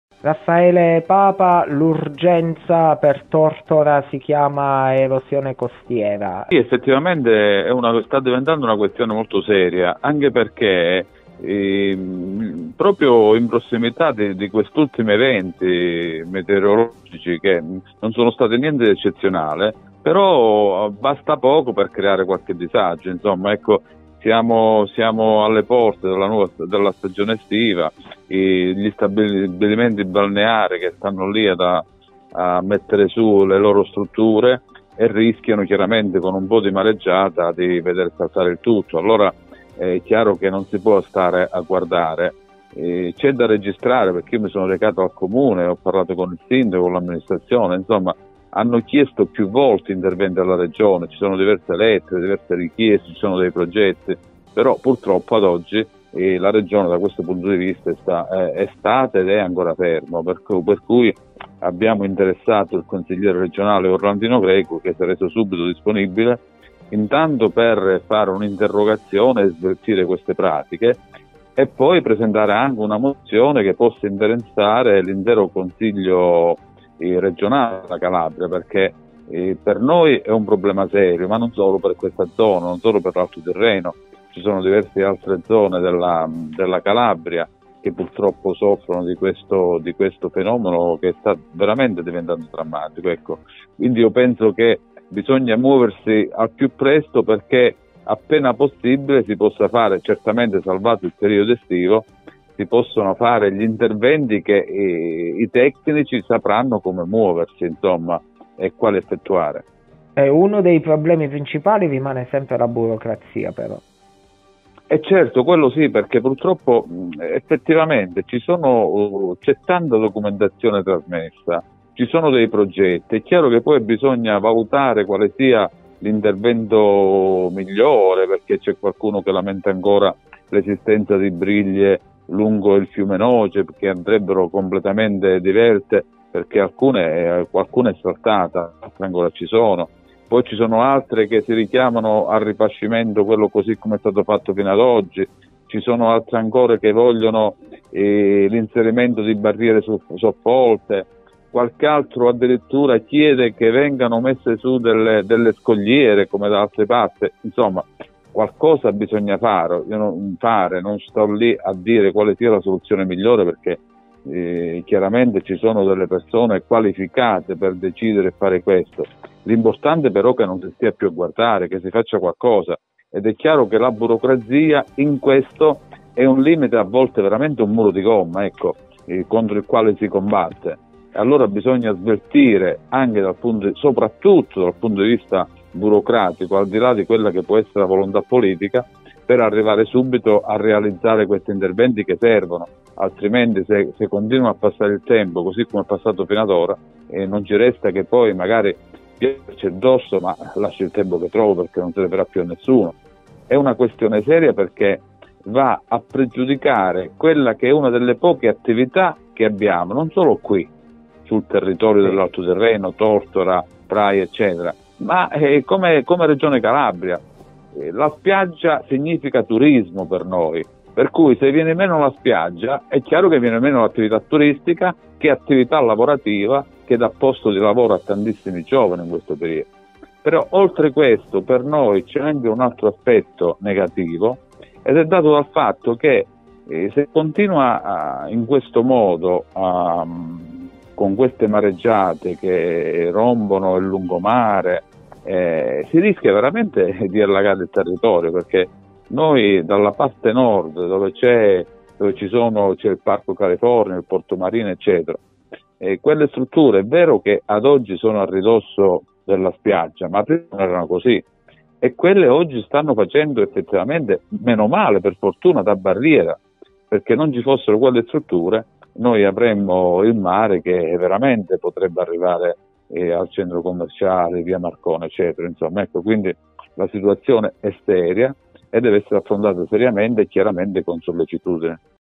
Erosione costiera a Tortora. Un'emergenza da risolvere
Erosione costiera a Tortora. L'emergenza del comune tirrenico in un'interrogazione del consigliere regionale, Raffaele Papa.